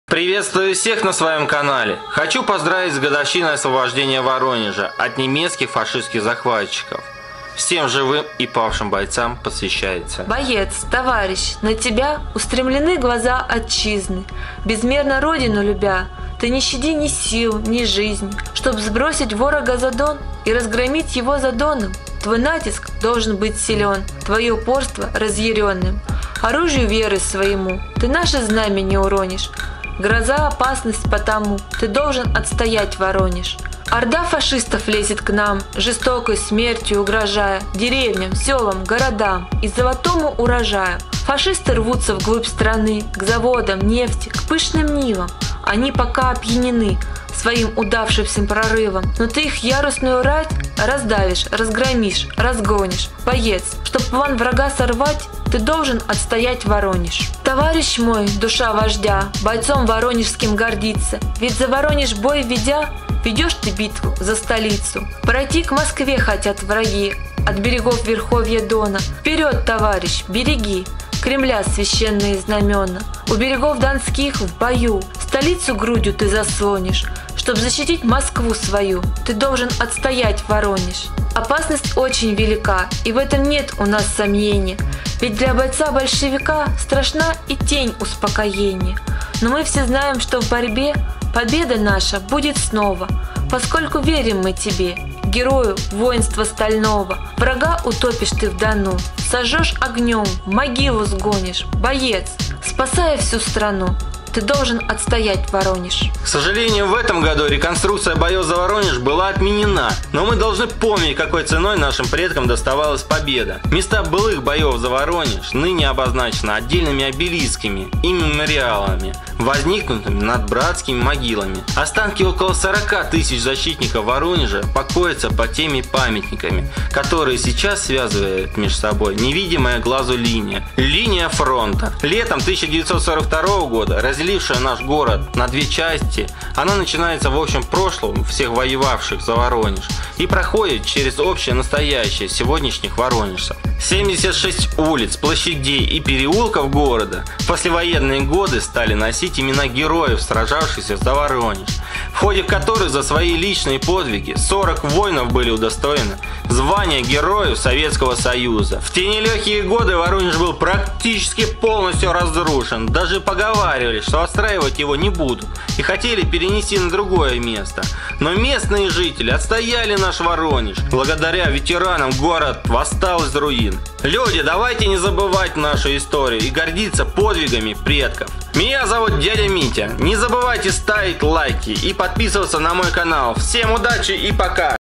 25 ЯНВАРЯ День освобождения Воронежа. JANUARY 25 Voronezh Liberation Day
25 января – День освобождения Воронежа от немецко-фашистских захватчиков
Дорогие ветераны! Уважаемые земляки!

Освобождение Воронежа было одним из ключевых этапов на пути к нашей общей Великой Победе, 75-летие которой мы будем отмечать в этом году. А для всех воронежцев 25 января – второй день рождения любимого города.